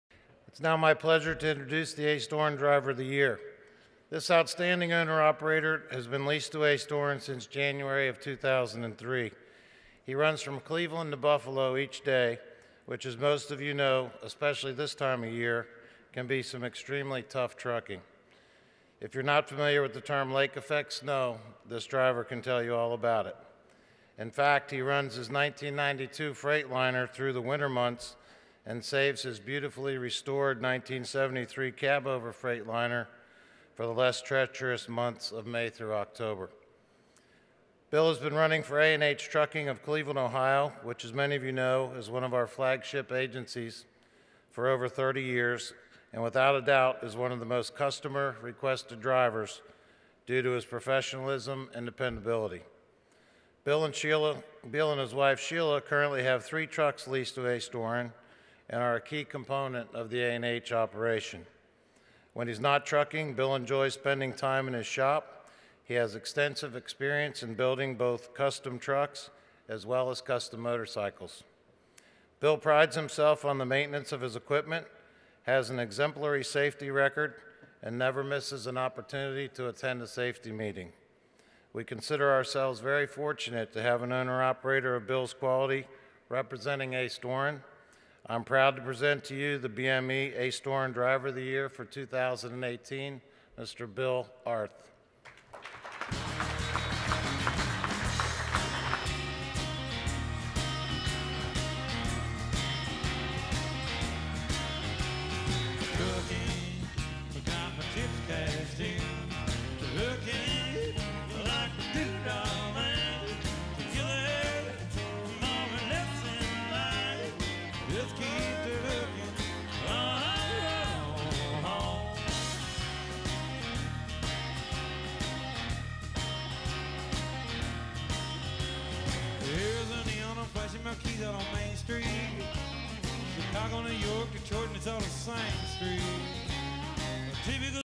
2018 Ace Doran Driver of the Year - William Arth
This distinguished award was given at Bennett's 2019 Annual Symposium at Atlanta Evergreen Marriott, Stone Mountain, GA. February 9, 2019. Congratulations William and Sheila.